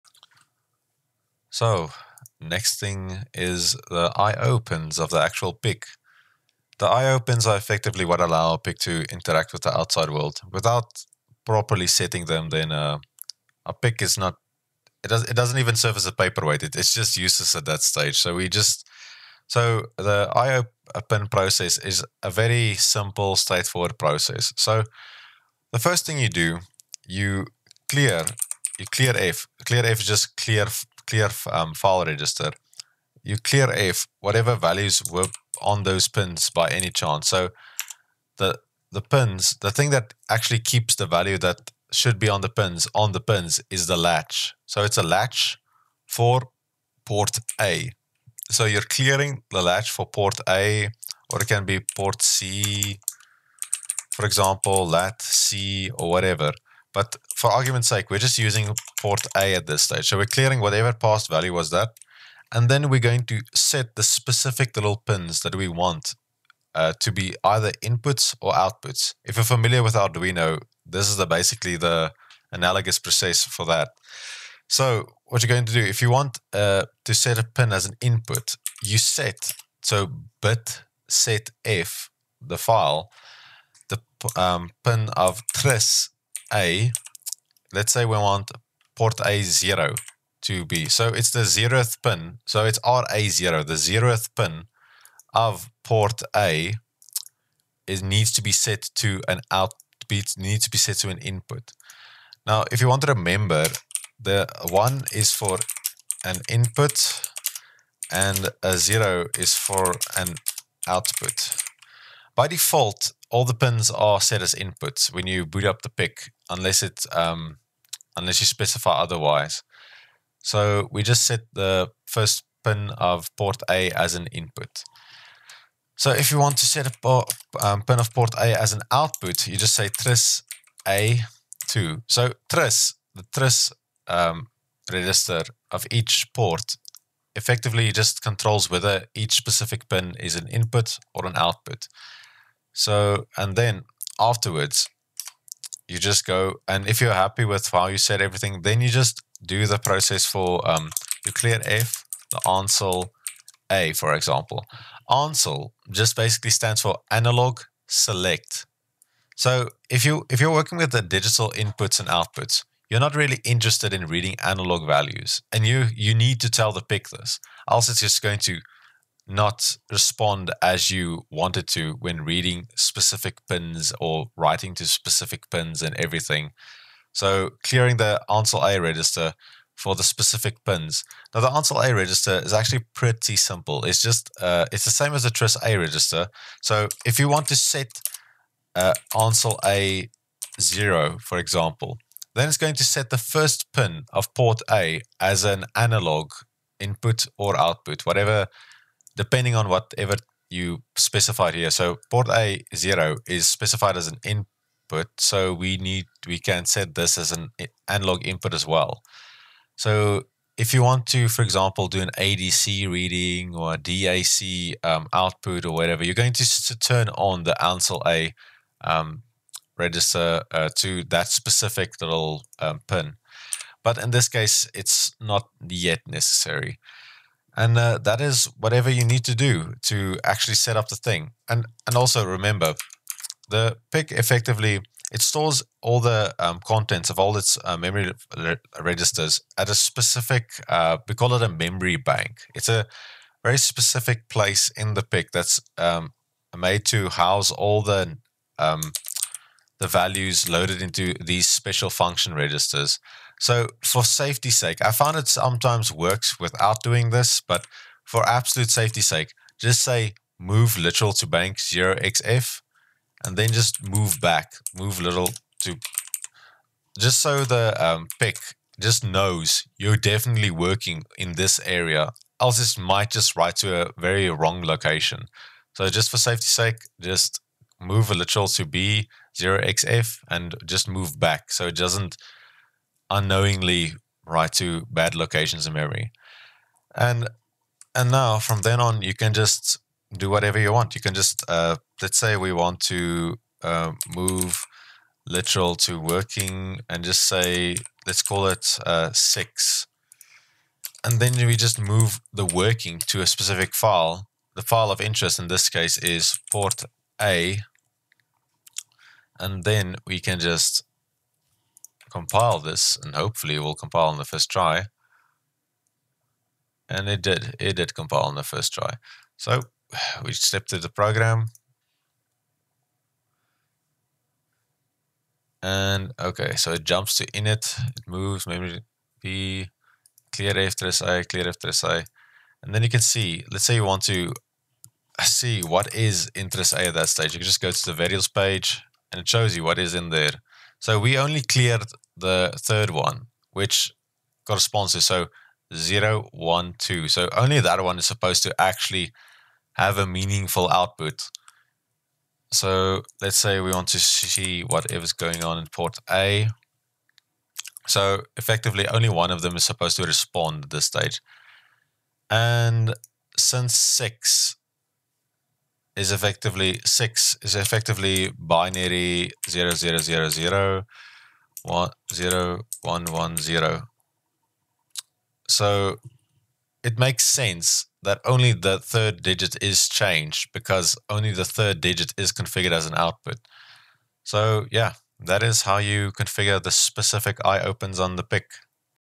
PIC18F45K22 - Input and Output Basics in Assembly
The magic of a microcontroller lies in generating the right outputs by reading certain inputs, and this video will enable you to do both.